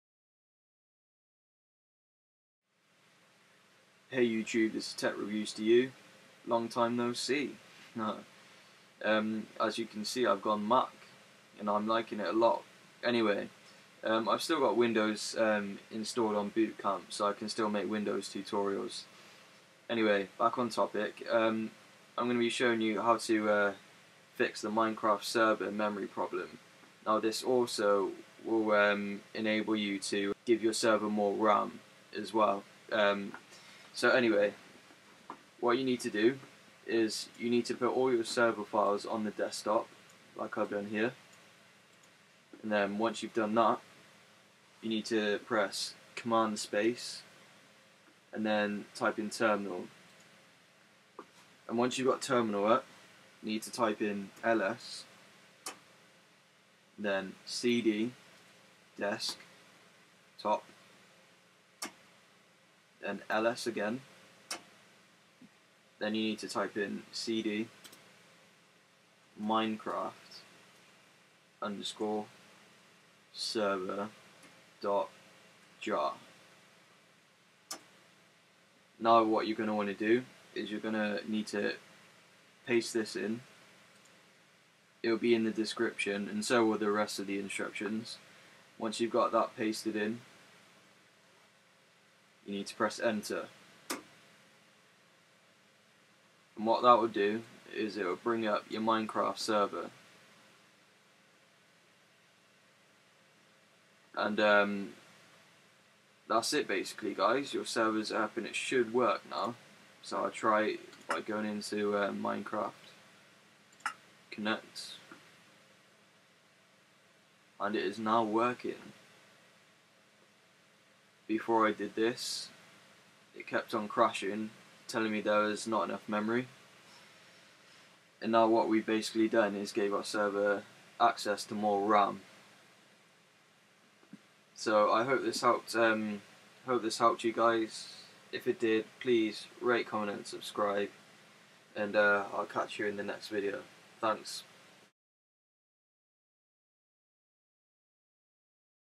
Minecraft Server Tutorial - How to give access to more memory + FIX
Hey all, this tutorial will show you haw to give more memory to your Minecraft server to stop it from crashing, lagging and in some cases, in order to make it play.

If you have the error code below, this should solve your problem:

Connection Lost

Internal exception: java.net.SocketTimoutException: Read timed out

OR

java.lang.OutOfMemoryError: Java heap space

Instructions:

1. Open Terminal, then type "ls"
2. Put all you server files on your desktop
3. Then go back into terminal and type in "cd desktop"
4. Then type "ls" again
5. Then type in "cd minecraft_server.jar"
6. You may get a error, but it's fine, just ignore it
7. Copy and paste in this: java -Xmx1024M -Xms1024M -jar minecraft_server.jar
8. Enjoy!